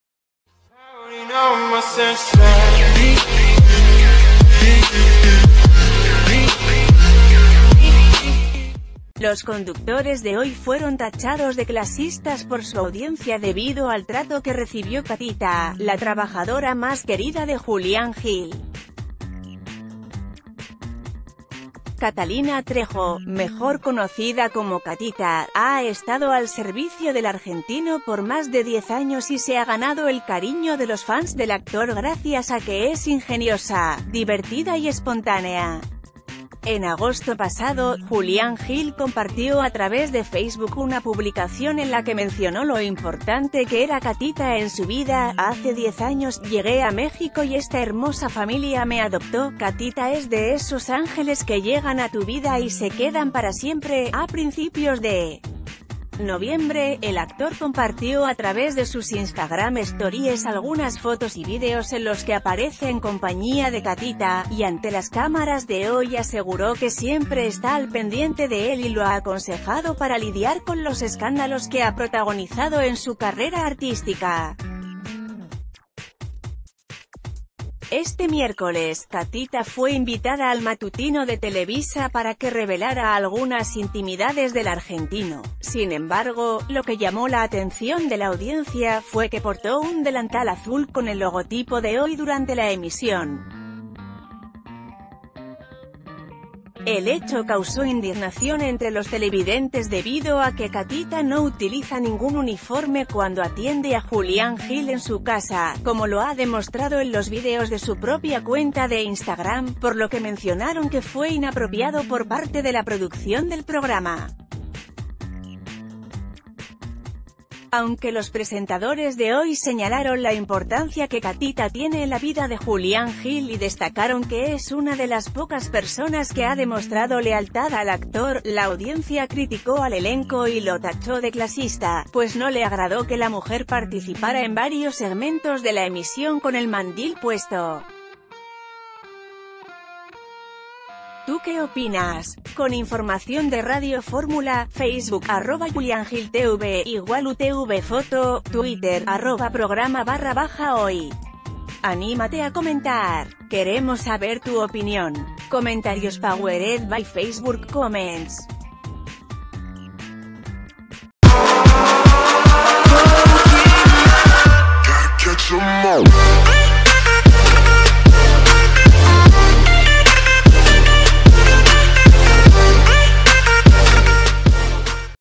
Critican a conductores de Hoy por trato clasista a trabajadora de Julián Gil
Los conductores de Hoy fueron tachados de clasistas por su audiencia debido al trato que recibió Catita, la trabajadora más querida de Julián Gil. Catalina Trejo, mejor conocida como Catita, ha estado al servicio del argentino por más de diez años y se ha ganado el cariño de los fans del actor graci...